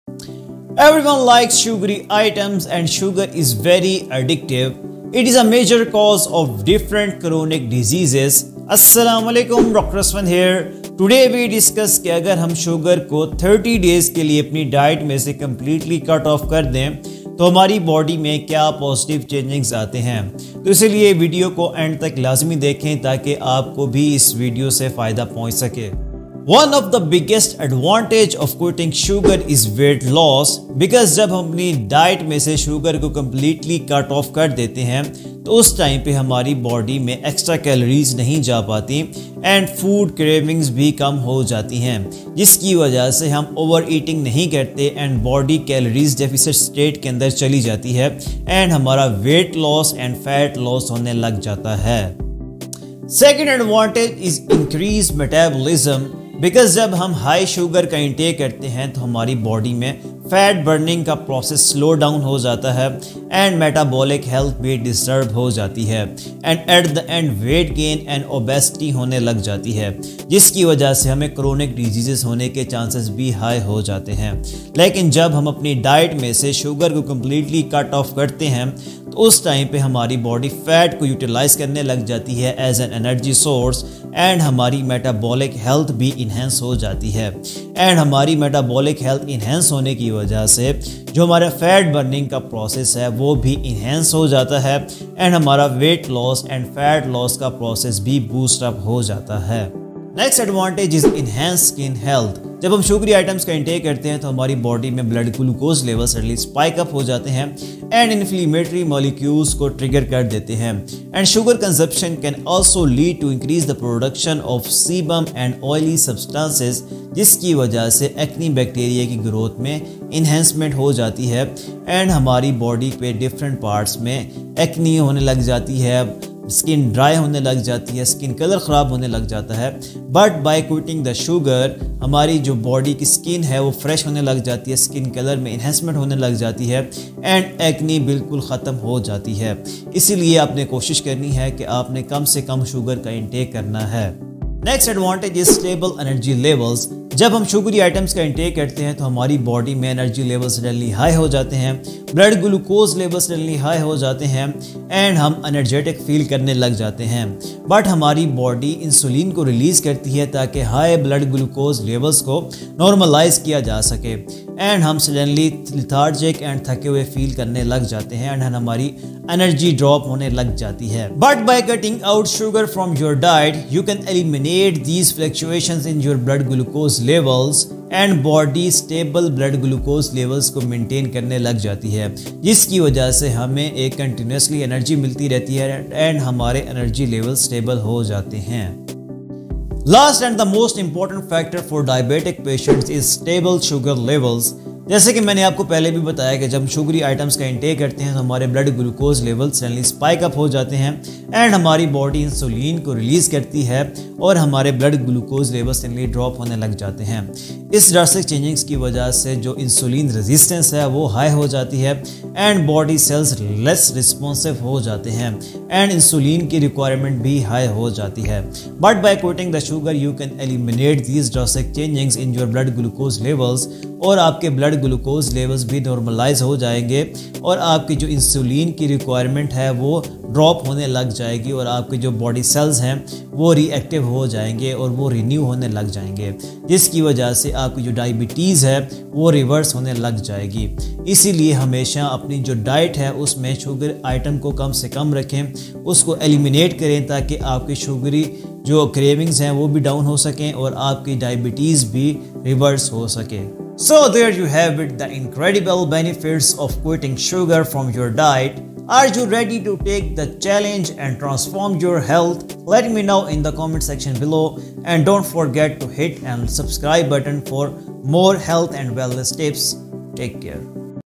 Quit sugar for 30 days and achieve the best shape of your body | Weight Loss | Fat Loss
Embark on a transformative journey with me as we explore the incredible benefits of quitting sugar for 30 days. In this eye-opening video, you'll witness firsthand the astounding effects of eliminating sugar from your diet, from shedding excess weight to sculpting your body into its best shape ever. Join me as I share valuable insights, practical tips, and inspiring success stories that will empower you to take control of your health and achieve the body of your dreams. Don't miss out on this life-changing experience - hit play and let's unlock your full potential together!

what happens if i quit sugar for 2 weeks
 what happens if i quit sugar for 30 days
 what happens if i quit sugar
Cheeni chorney ky faidy